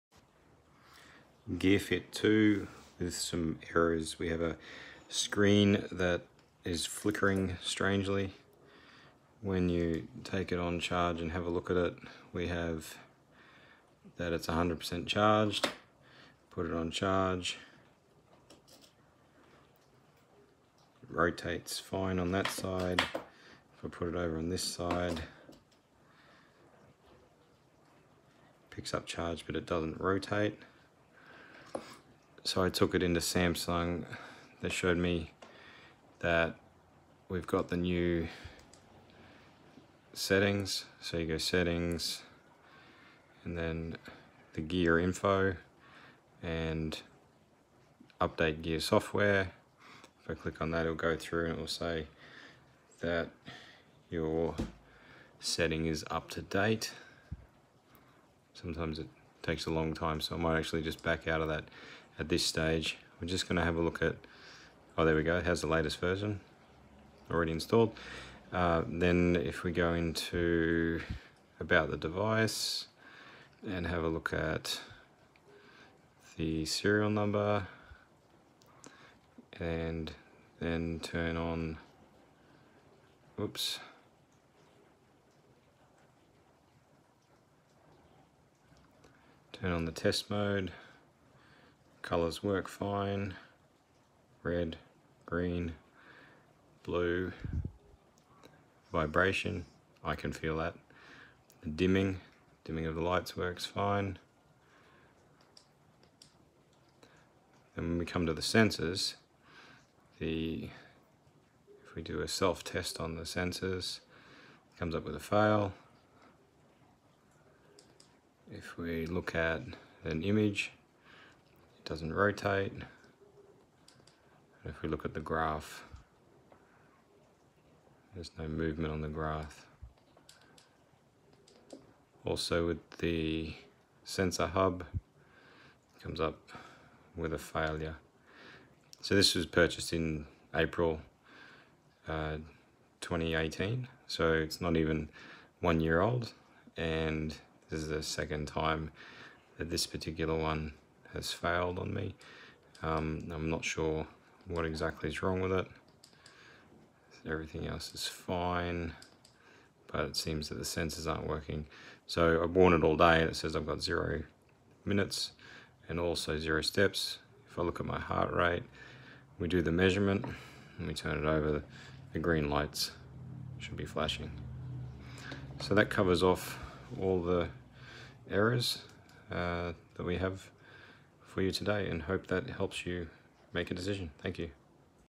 Samsung Gear Fit2 Pro Service Menu Test Mode
Gear Fit2 Pro error message how to find the service menu

Gear Info
About Device

and tap 5 times on the software version, you get a prompt for a PIN code

*#0*# - Test Mode

Can test the sensors, screen colours brightness.

I found my sensors have malfunctioned.